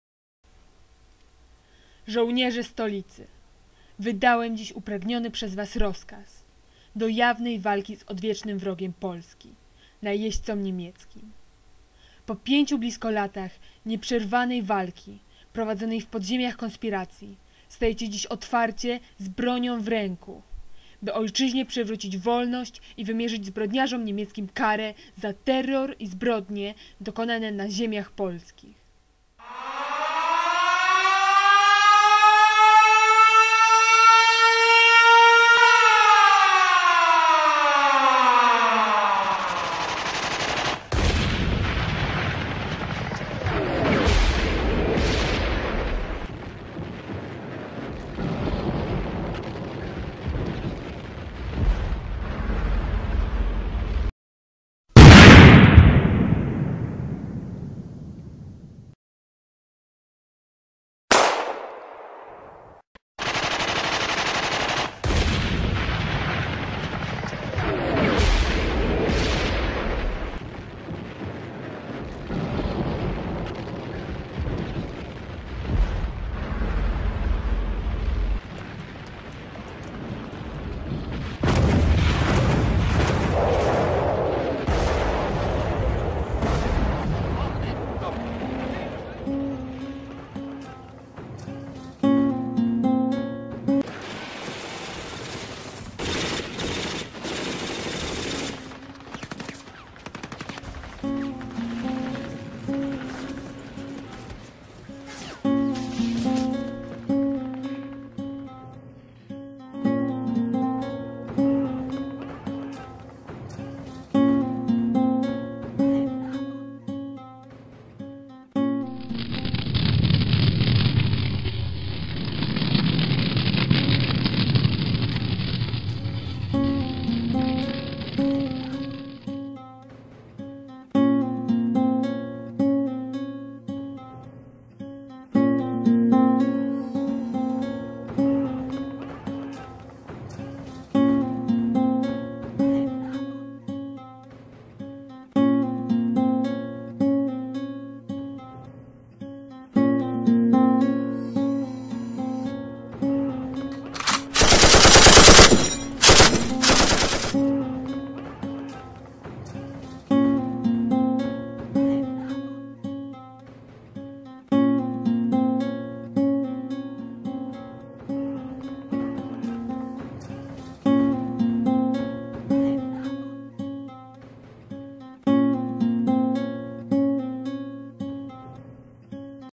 Ochota z popiołów powstała
Film jest retrospekcją zdarzeń z czasów Powstania Warszawskiego 1944 r. na Ochocie. Bohaterowie filmu to sanitariuszka, powstaniec oraz łączniczka.
Autorki: Kamila Prętkopowicz, Oliwia Jaroszewska, Basia Sobieszek, Karolina Banaśkiewicz,
Gimnazjum nr15 im.gen.dyw.Stefana Roweckiego "Grota"